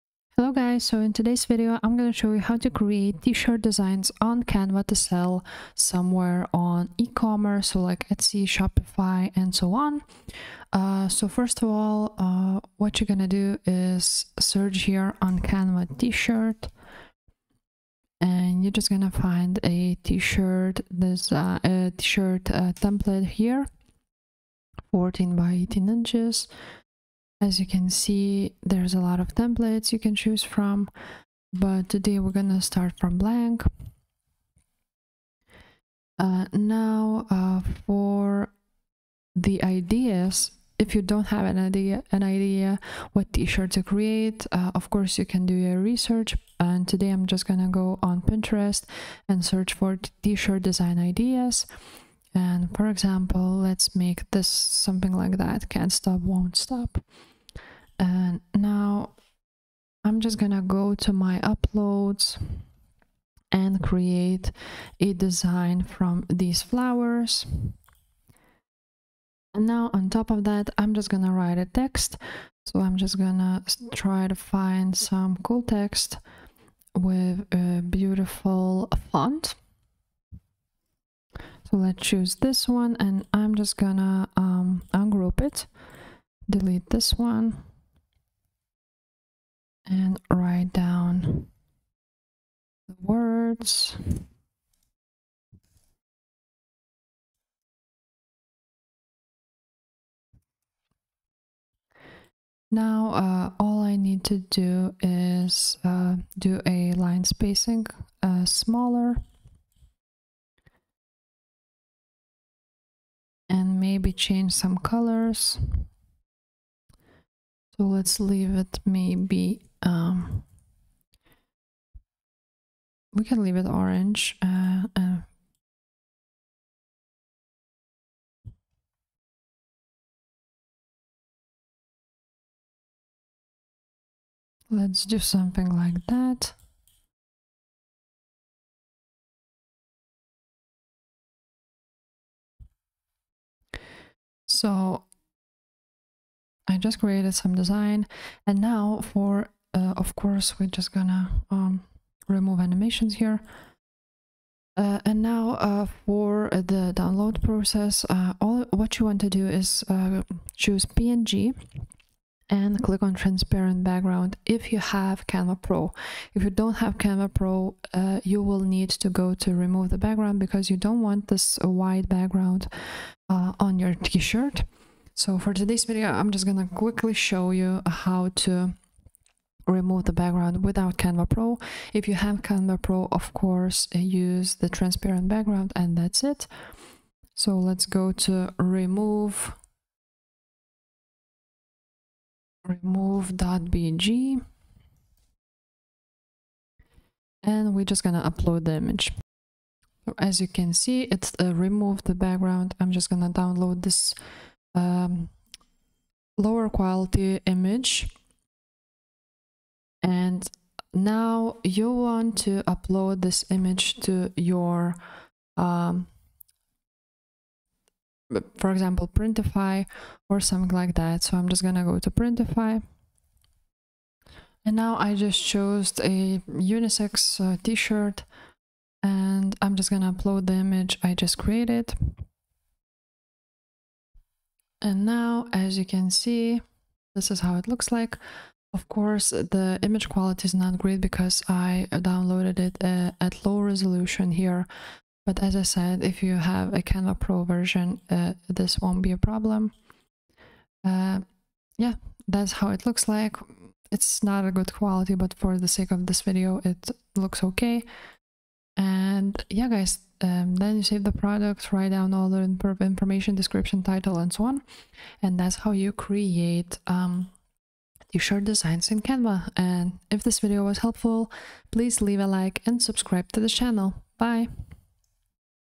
Create T-shirt Designs On Canva To Sell | Canva & Ecommerce
In this video I show you how to create t-shirt designs on Canva to sell them on ecommerce stores!